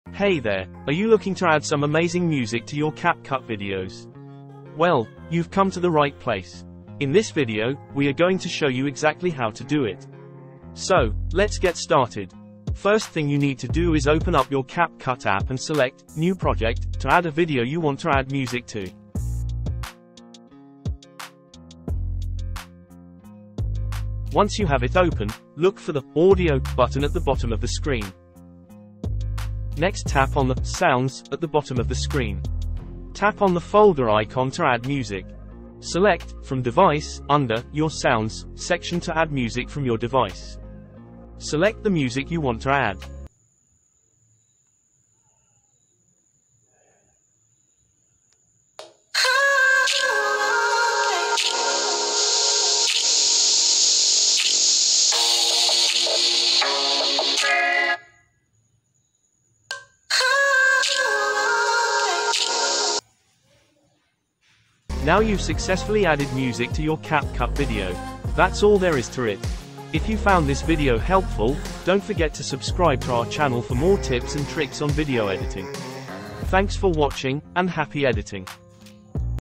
How to add music to Capcut | Capcut edit tutorial
How to add music to capcut. This capcut tutorial will help you to add music or audio in capcut editing app.